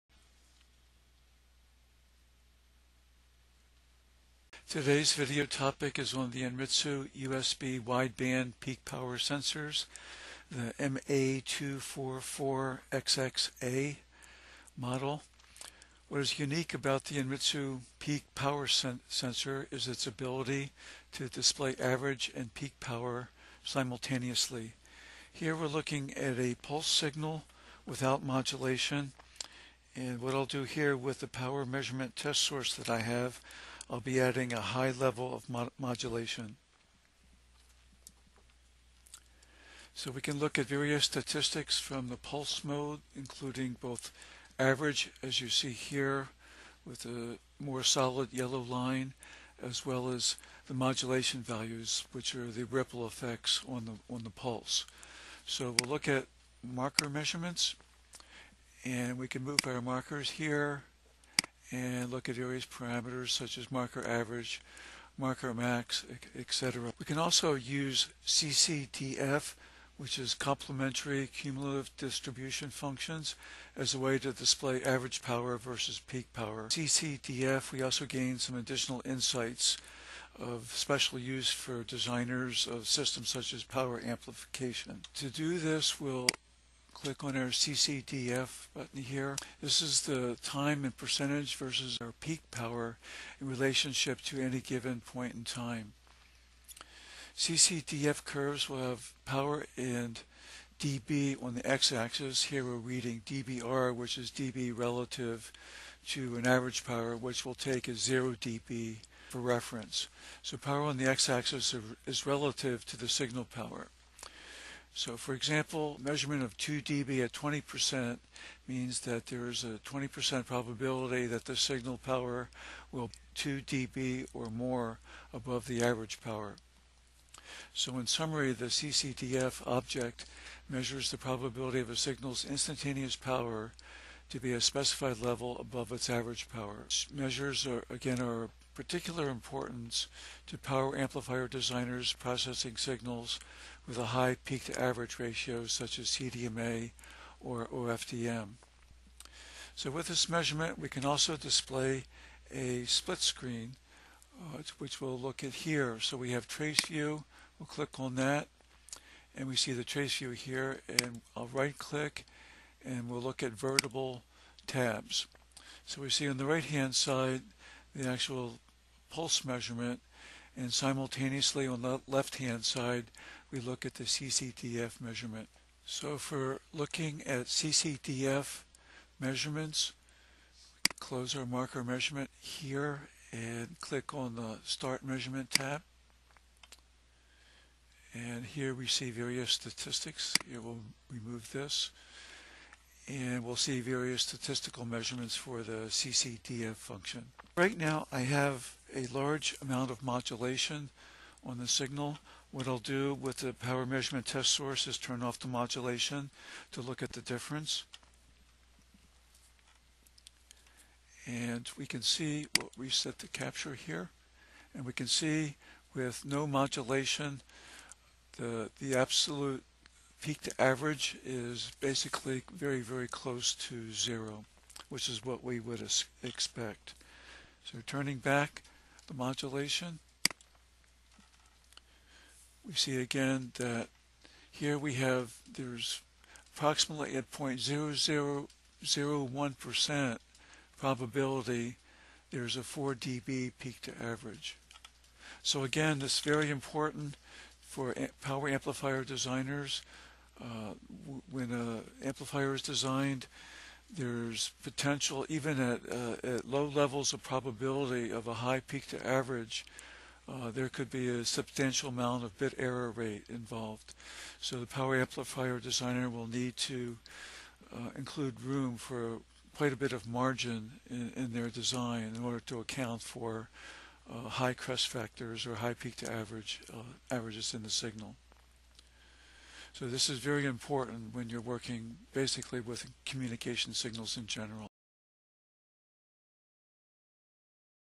Conducting CCDF Measurements with Anritsu MA244xxA Peak Power Sensors
This video shows how to conduct CCDF measurements with the Anritsu MA244xxA wideband peak power sensors. CCDF is a way to display the percentage of transmitting certain power levels as a function of probability.